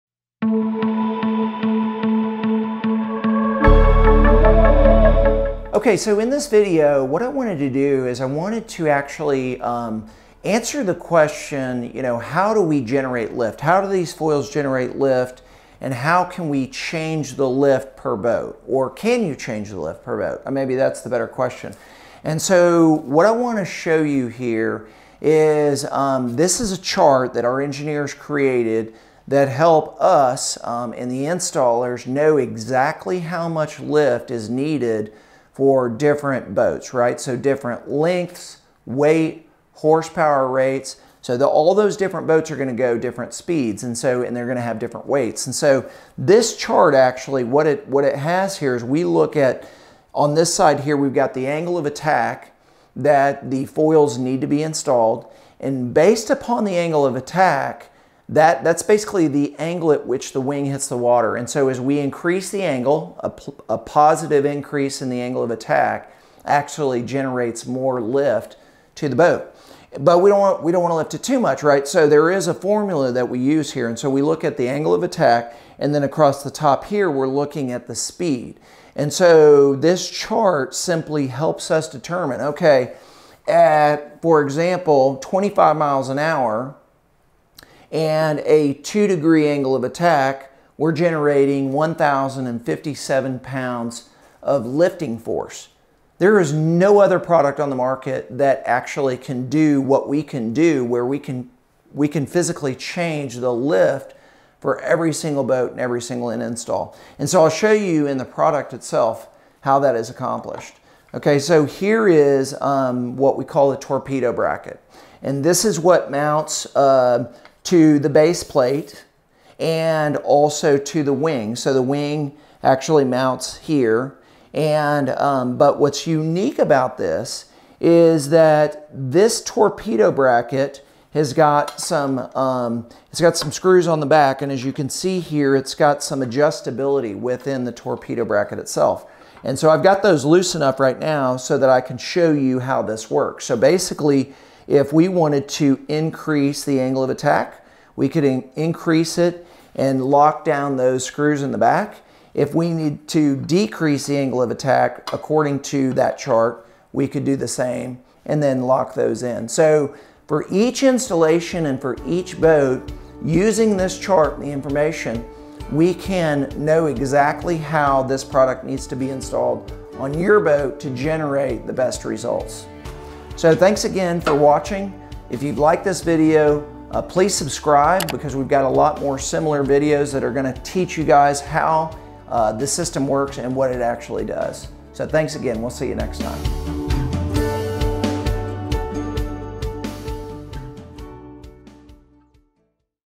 Hydrofin creates the pontoon lift you need - What determines the lifting force?
You love lounging on your pontoon, but you have a need for speed. The best way to marry your desire for pontoon boat comfort and speed boat excitement is with a hydrofoil lift. The Hydrofin system is a pontoon boat accessory that lifts your boat, which increases speed and makes your ride fast and fun.

The Benefits of Hydrofoil Lift

Hydrofin is customized to your double pontoon boat. Engineering professionals have determined the perfect install for each double pontoon regardless of size. The Hydrofin team uses the weight, length and horsepower rate of your boat to create a perfect fit. All you have to do is talk to the Hydrofin team to learn exactly how this product will increase your enjoyment every time you take your pontoon to the river or lake.

The Hydrofin Angle of Attack

The foils are installed at a specific angle to create the lift you need for higher speeds. The angle of attack refers to the angle at which the wing hits the water. This angle varies depending on the size and weight of your double pontoon boat. The more positive the angle is, the more height your boat will reach. Higher heights will equal faster speeds. At the same time, too much lift can be counterproductive. Hydrofin offers a team of experts who understand how to perfect hydrofoil lift for every double pontoon.

A One-of-a-Kind Product for Your Recreational Needs

The Hydrofin difference is the ability to customize the fit of the wing. No other pontoon accessory like this has a fit that is tailored to each boat. This is accomplished through use of an adjustable torpedo bracket. That bracket mounts to a base plate on the pontoon boat. The hydrofoil wing is then mounted to the torpedo bracket. The bracket itself is installed at the precise angle determined based on your double pontoon boat's specifications.

Contact Hyrodfin today for a lighter, quicker and more entertaining ride.